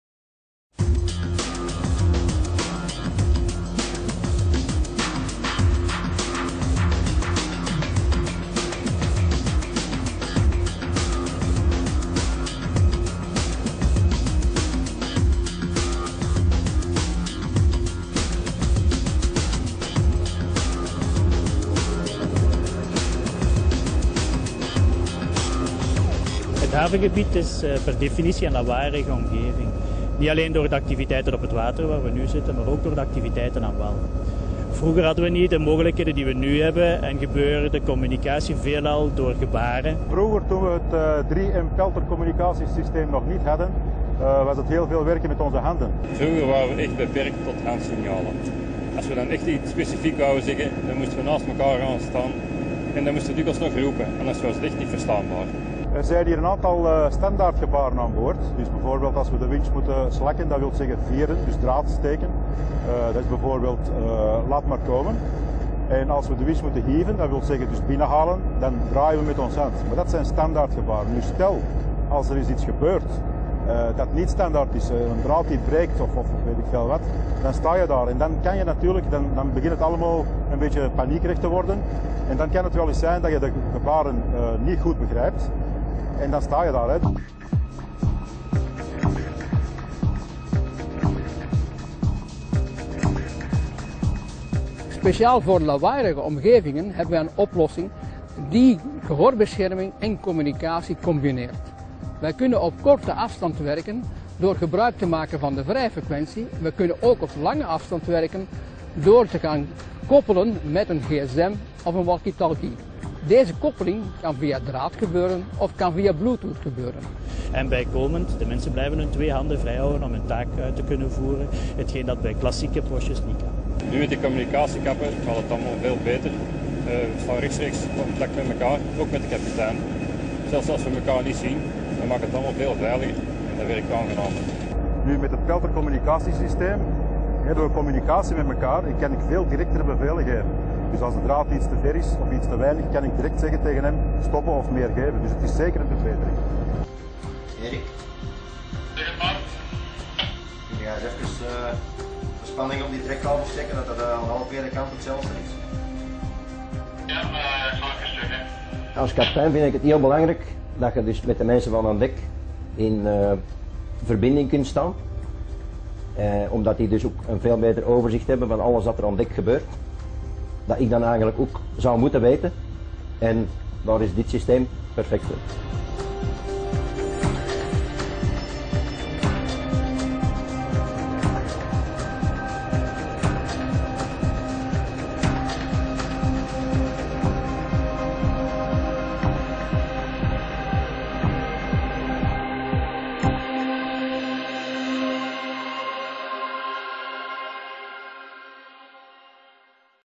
Je hoort het goed gehoorbescherming en communicatie in één
De nieuwe gehoorkappen van de 3M™ Peltor™ X-serie koppelen een uitstekende bescherming aan het ultieme draagcomfort. De gehoorkappen van de 3M™ Peltor™ X-serie maken het eenvoudiger om zelfs de meest lawaaierige werkplaatsen het hoofd te bieden. U zult amper merken dat u ze draagt!